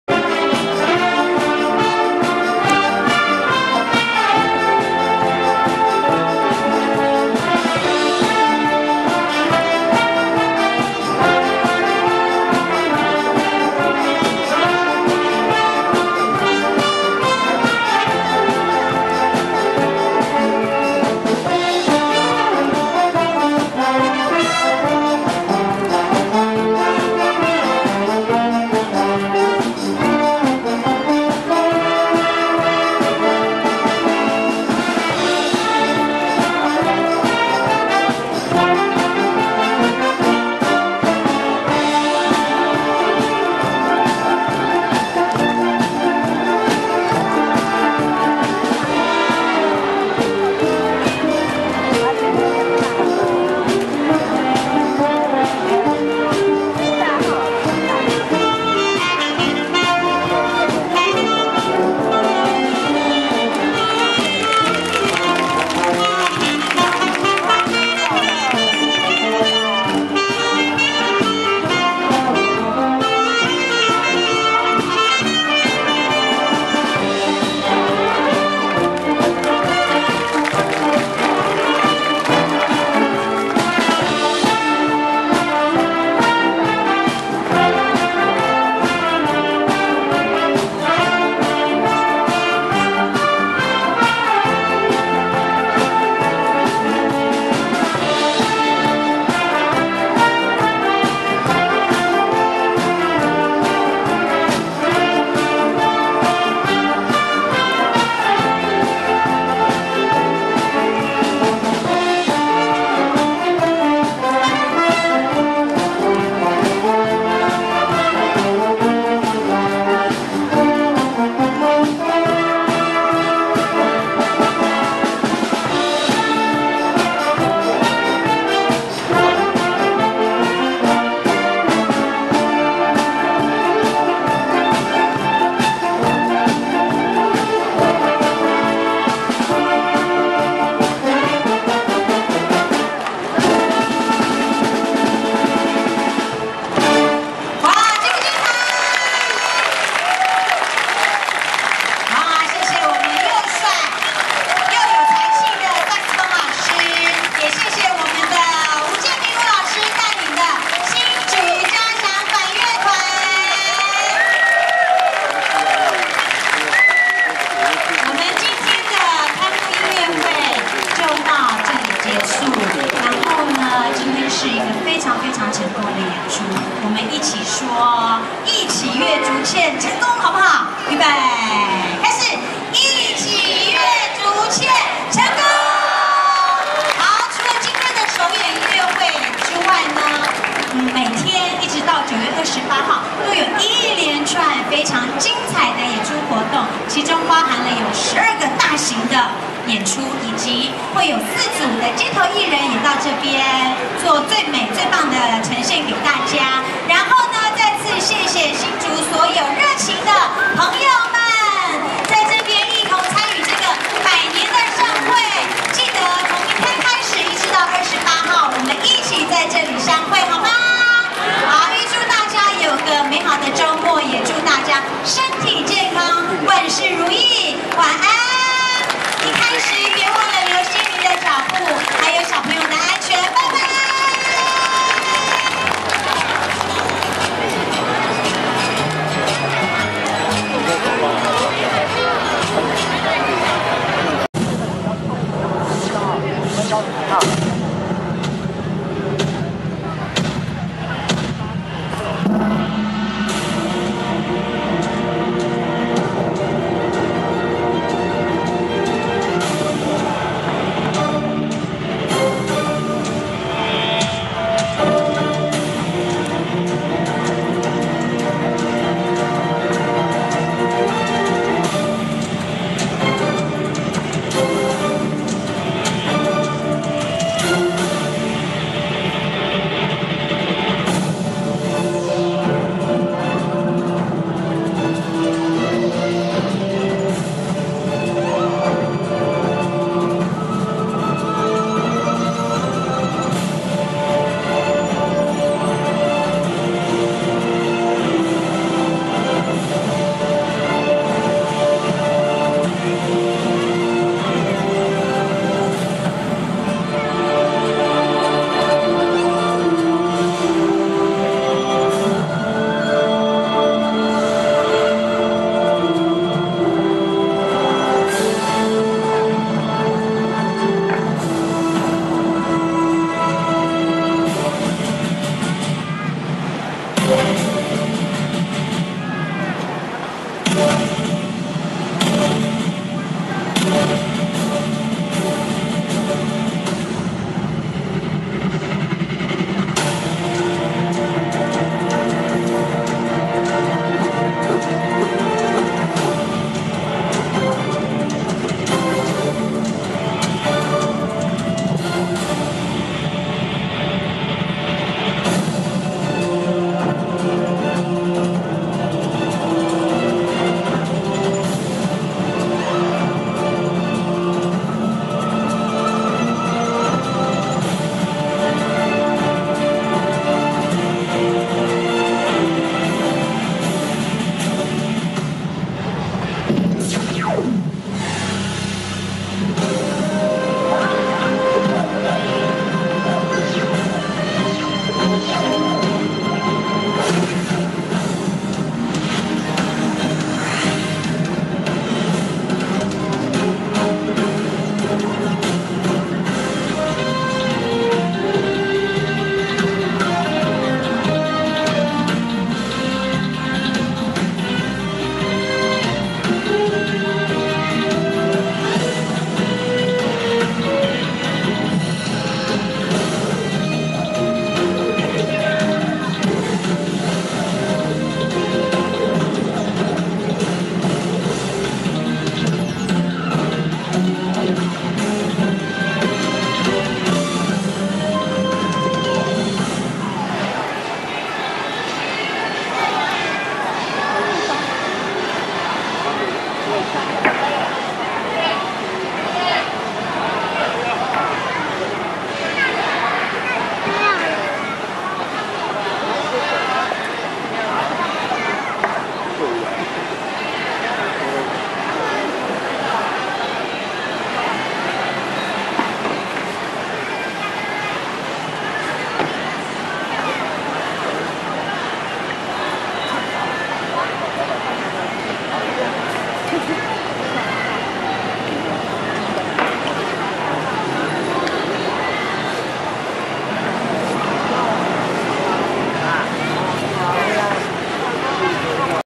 103年9月13日（週六）2014幻影光雕東門城、藝起樂竹塹--185週年城慶活動之一。
「幻影光雕東門城」新竹市竹塹城迎曦門185週年城慶系列活動之一，夜晚的東門城仍是厚實而充滿古意的，靜靜地在夜空中矗立昂揚！